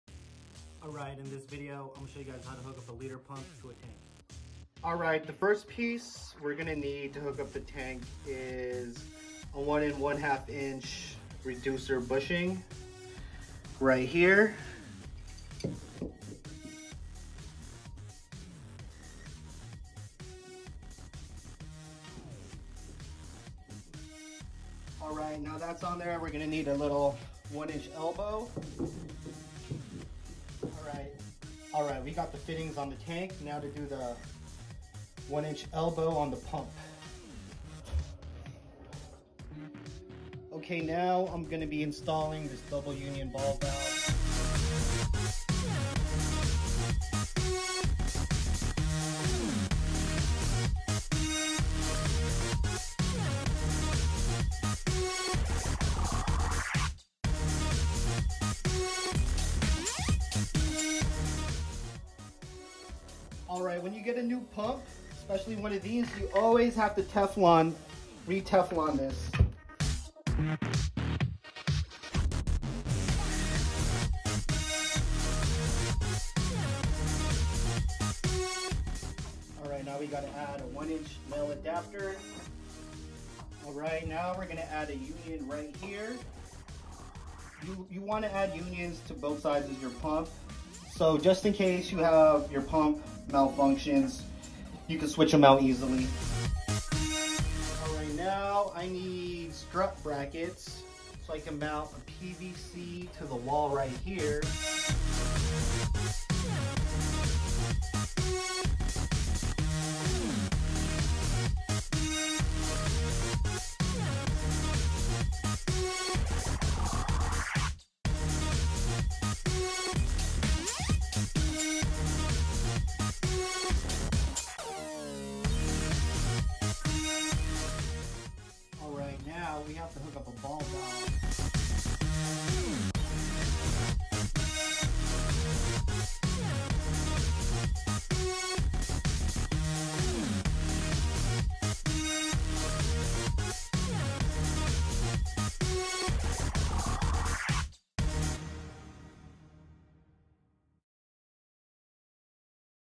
Hydroponic Drip Install Part 1
Hooking up a Leader pump to a tank.